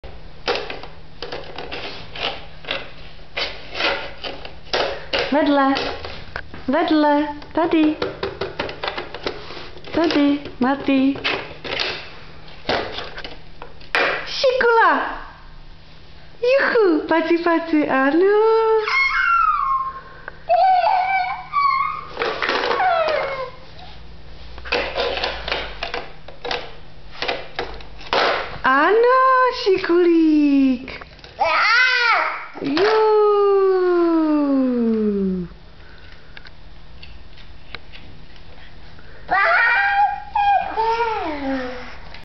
Matý a vkládačka
ještě to není úplně ono a mamka mi musí hodně pomáhat, ale lepším se každým dnem... :-)